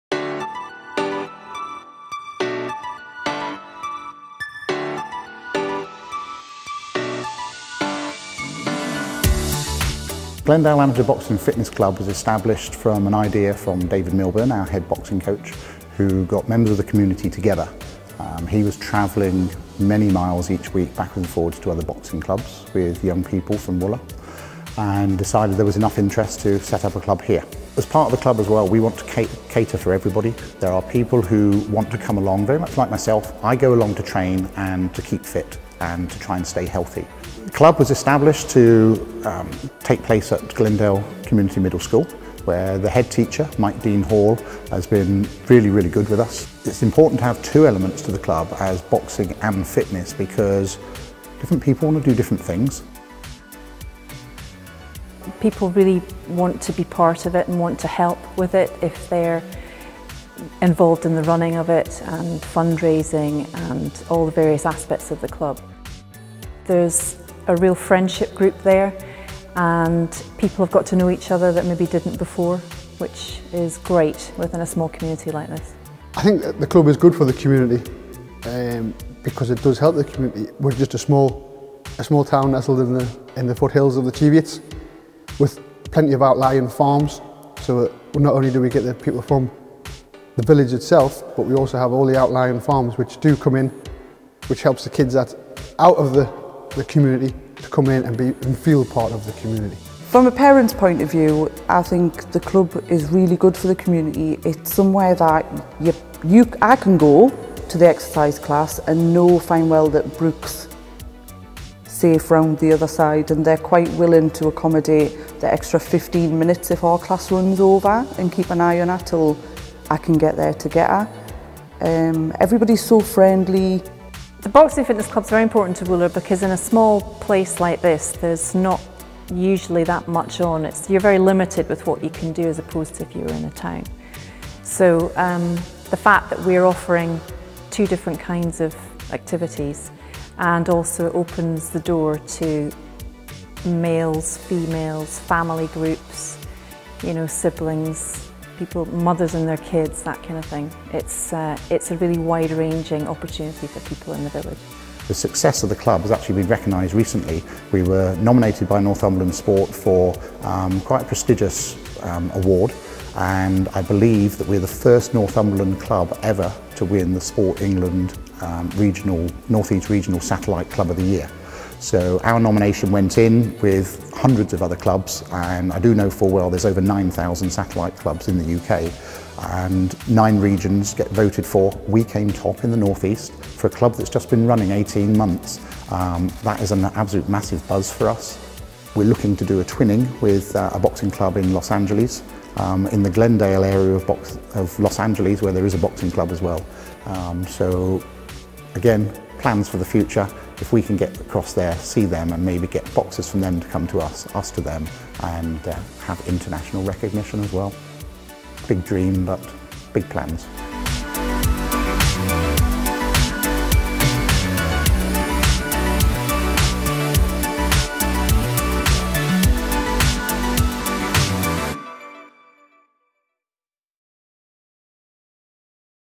Glendale Amateur Boxing & Fitness Club - Satellite Club
What a club!  The Glendale Amateur Boxing & Fitness Club truly has captured the community spirit to develop a club for all.

The club was awarded the Sport England North East Satellite Club of the Year Award in 2016 and Northumberland Sport are proud to be a part of it's success.